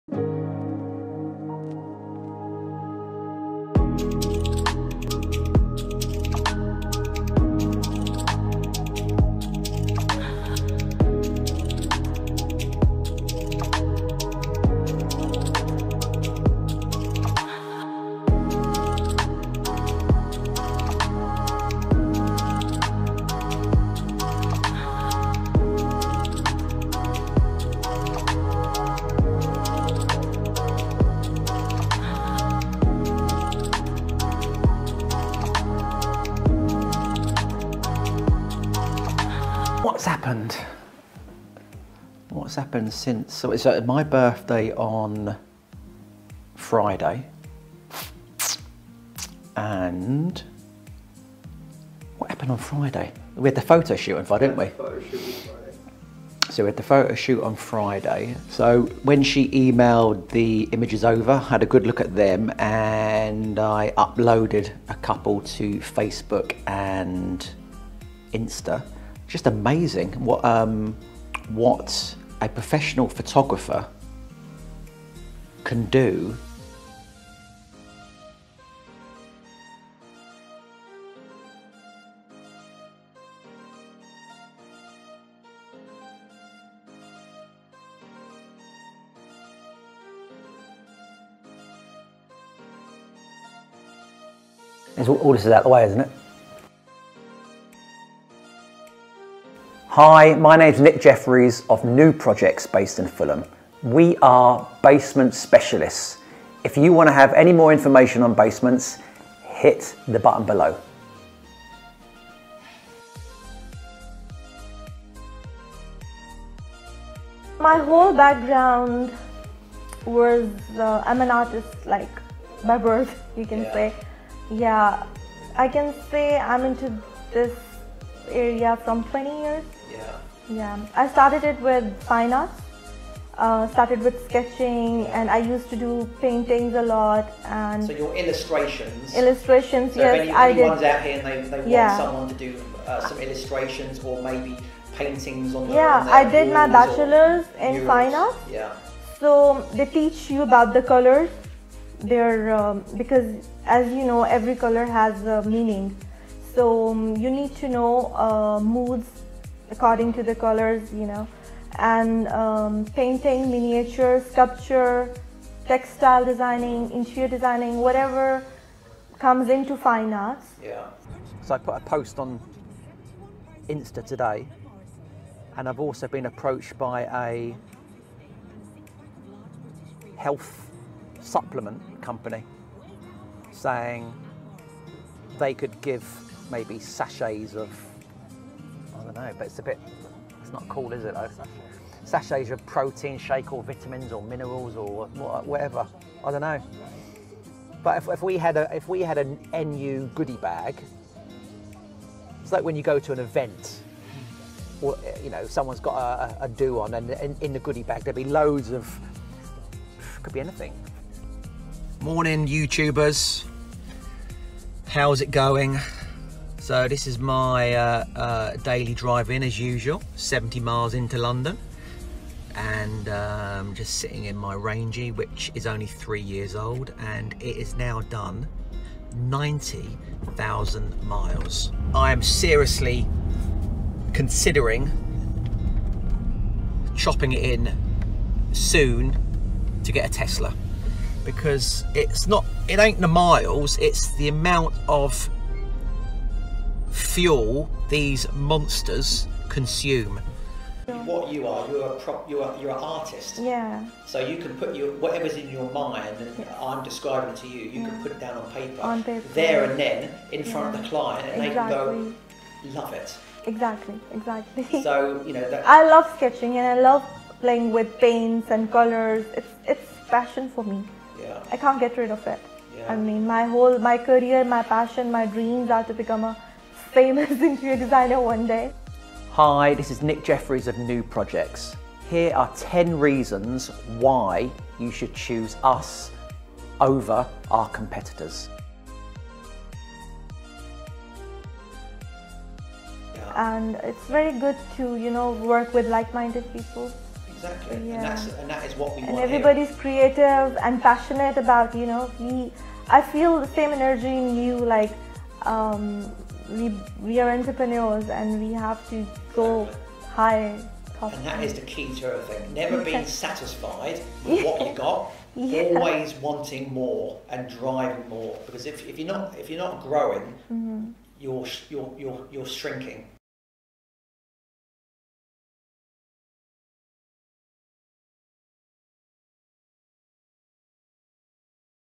How To Run A Successful Design & Build Company - Nu Projects - April 2019 Highlights
Nick Jeffries
My Youtube channel is documenting my life at home and at work.
Todays Episode are highlights from April 2019.
How To Run A Successful Design & Build Company

Nu:projects ltd
10-12 Fulham High Street
Fulham
London
SW6 3LQ